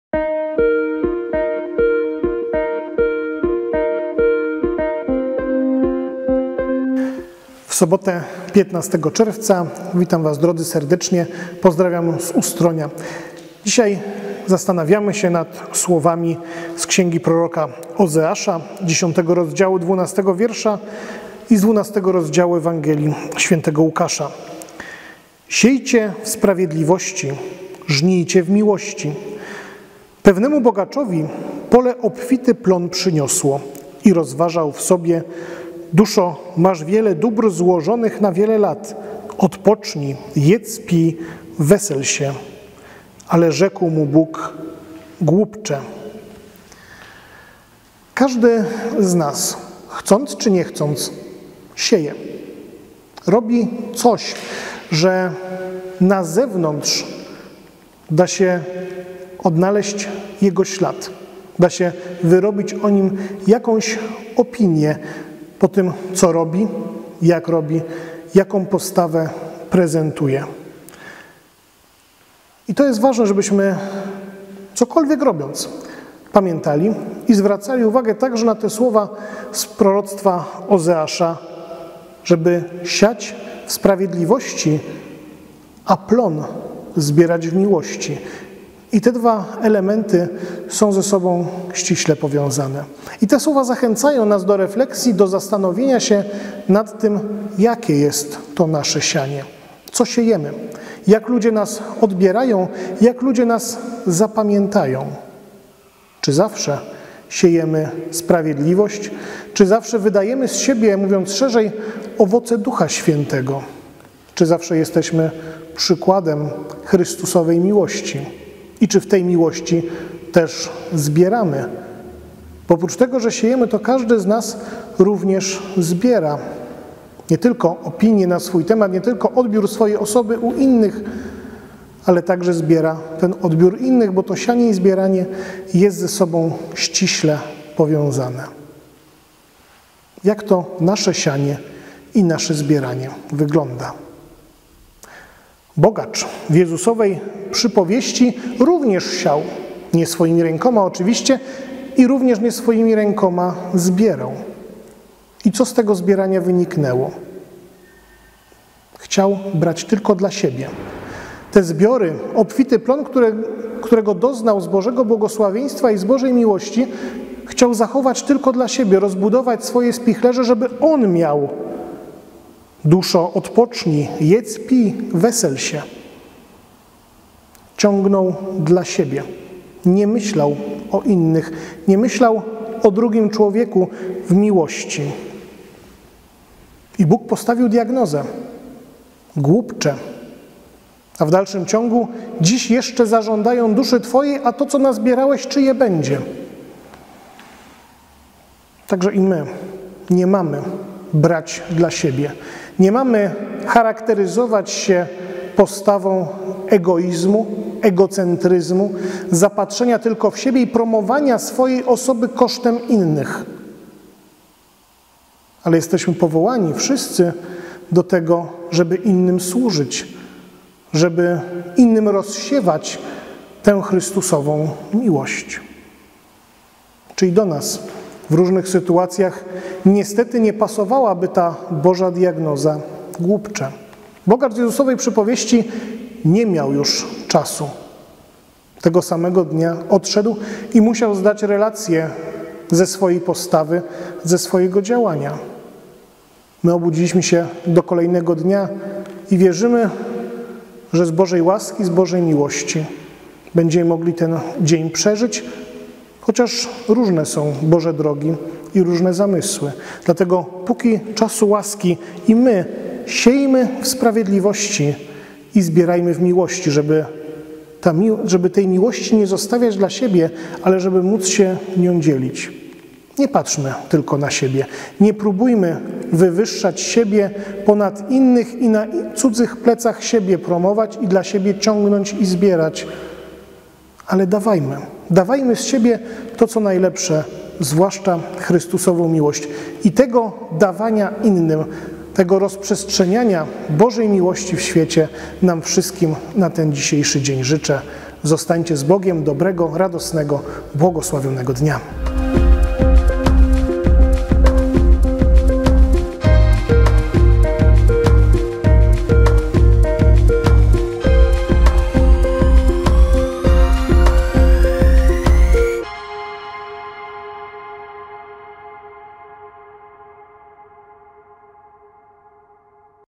Codziennik biblijny, Słowo na dzień 15 czerwca 2024 r.
ks. Zbigniew Obracaj, Parafia Ewangelicko-Augsburska w Ustroniu
Rozważanie biblijne na Słowo dnia:

Siejcie w sprawiedliwości, żnijcie w miłości.
Księga Ozeasza 10,12

Pewnemu bogaczowi pole obfity plon przyniosło. I rozważał w sobie: (…) Duszo, masz wiele dóbr złożonych na wiele lat; odpocznij, jedz, pij, wesel się. Ale rzekł mu Bóg: Głupcze.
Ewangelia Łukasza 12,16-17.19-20
Codziennie o 7 rano spotkanie z Biblią.